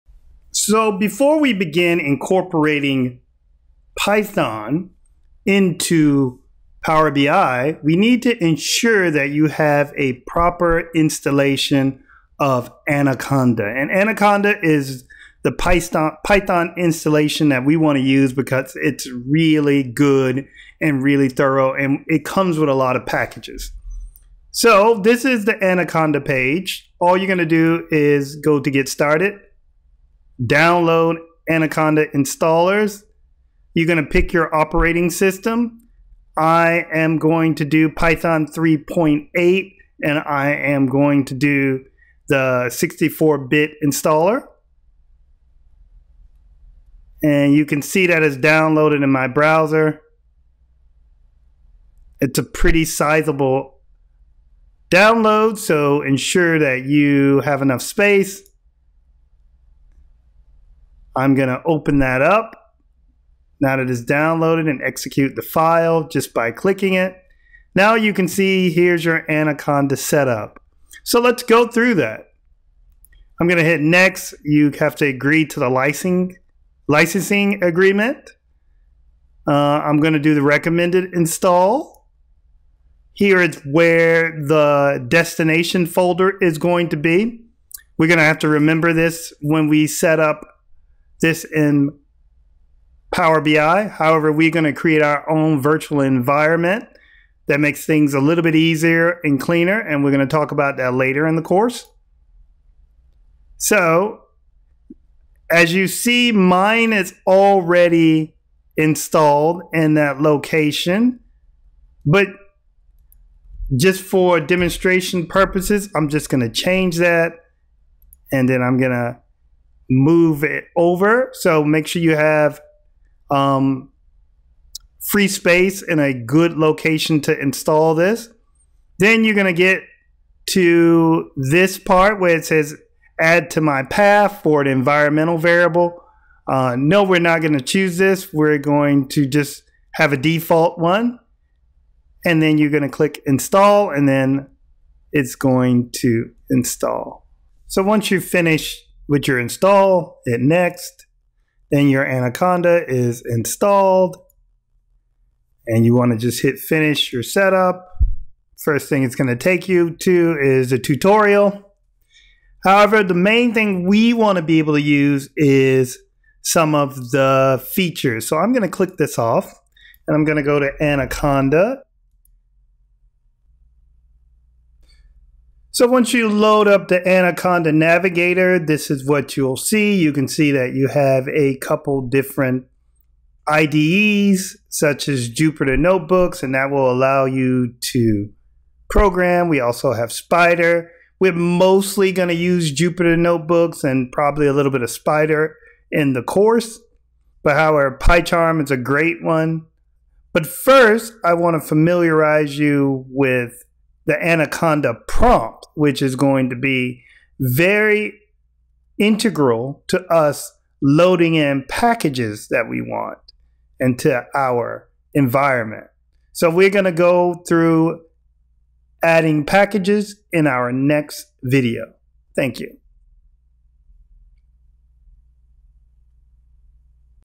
Power BI and Python Machine Learning A to Z Part 1: Installing Python
This is the first part of  the Power BI and Python series. You will be building your environment through installation of Anaconda. This is the most popular application for installing Python due to its ease of use and update schedule. You can find the link below for the the installation.